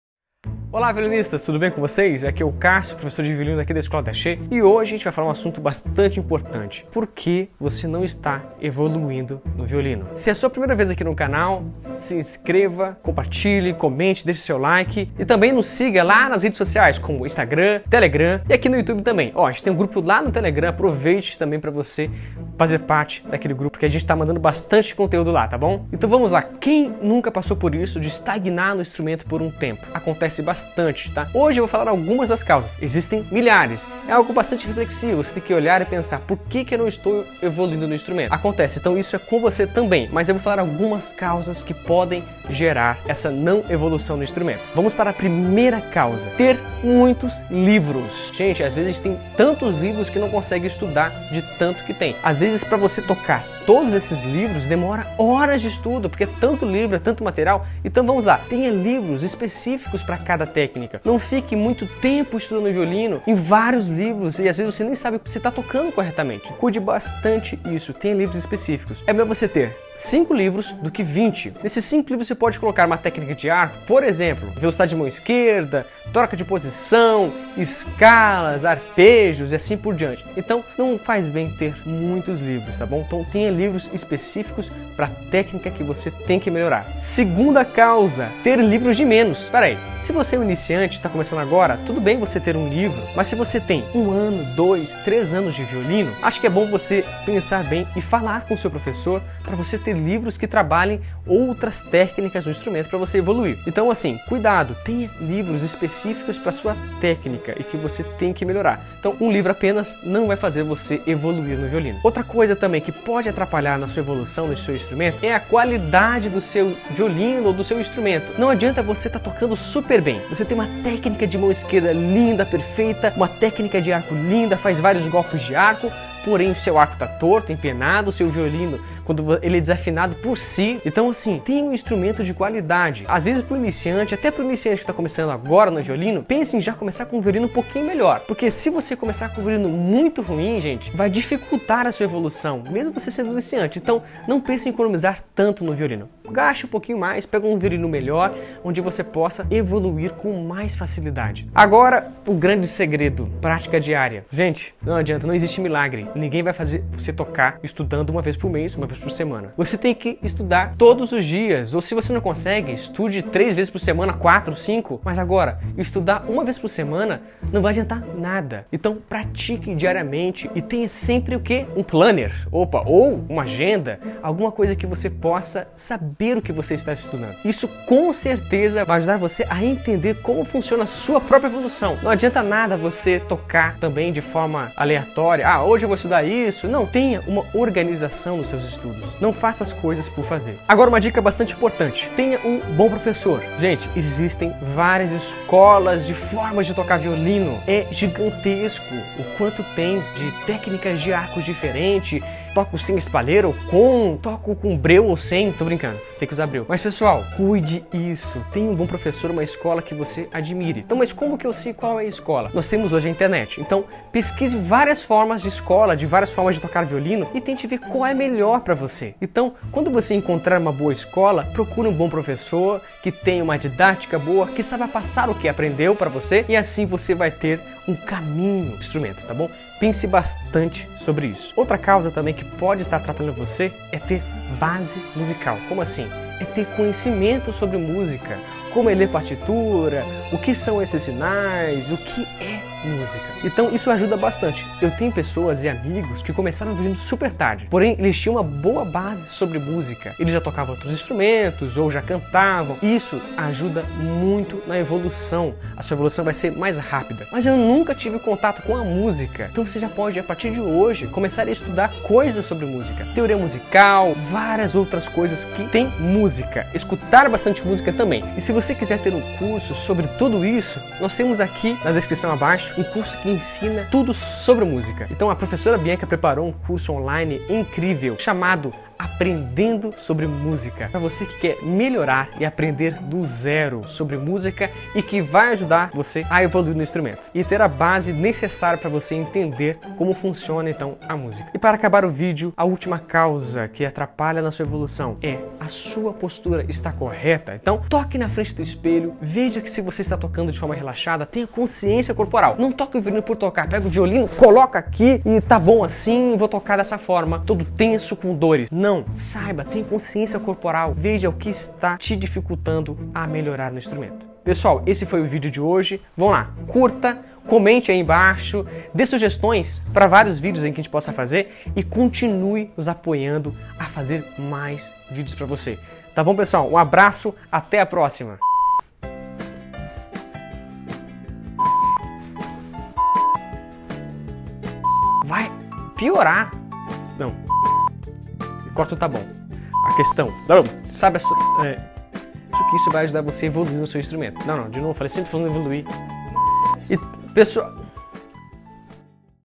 Porque não estou evoluindo no violino? | VEJA O QUE VOCÊ PODE FAZER PARA MELHORAR NO VIOLINO
Nessa aula para violinistas o professor Cássio do canal de violino D'archet preparou um tutorial de porque você não está evoluindo no violino, algumas dicas para evoluir no violino, passos para aprender melhor violino, como conseguir melhorar no violino. Muitas vezes a evolução no violino está ligada 100% a consciência no instrumento.
Será mesmo um segredo para conseguir evoluir no instrumento? Será que estar estagnado no instrumento é só para iniciantes no violino ou pessoas mais avançadas também precisam de técnicas específicas, exercícios específicos e mais!!